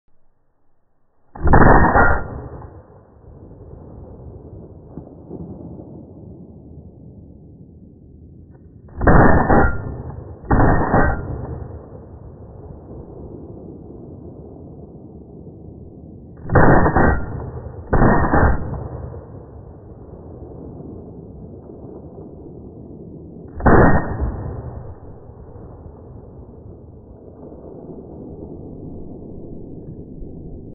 X-Pre 2 Slow Motion
Slow motion footage of the Huldra X-Pre 2’s low mass bolt carrier smooth cycling, quick rate of fire, and clean loading and extracting.